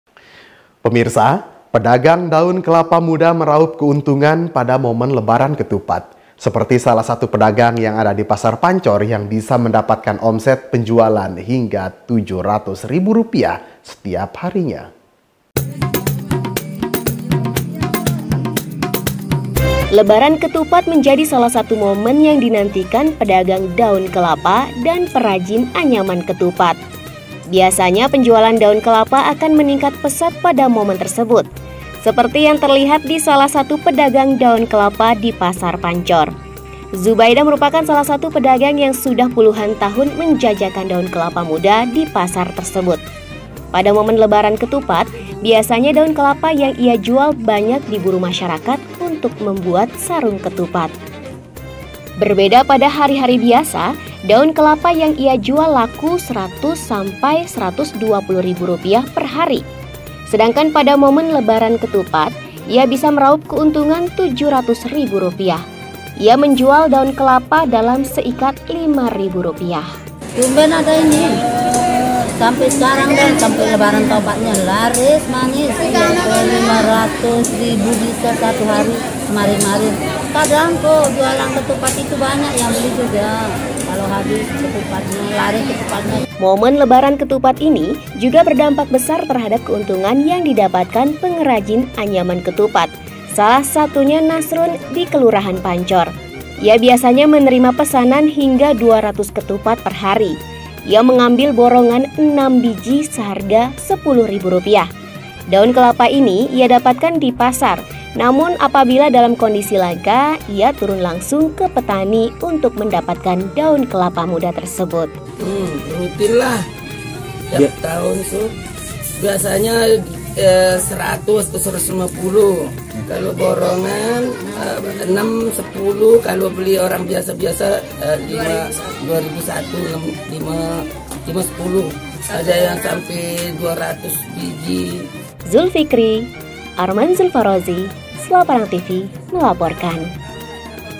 LEBARAN KETUPAT, PEDAGANG DAUN KELAPA RAUP UNTUNG
Pedagang Daun Kelapa Muda Meraup Keuntungan Pada Momen Lebaran Ketupat. Seperti Salah Satu Pedagang Yang Ada Di Pasar Pancor, Yang Bisa Mendapatkan Omset Penjualan Hingga 700 Ribu Rupiah Per Hari.

Berita ini menjadi salah satu yang kami turunkan dalam program berita Jendela Selaparang edisi Senin, 01 Mei 2023 disamping berbagai kabar menarik lainnya.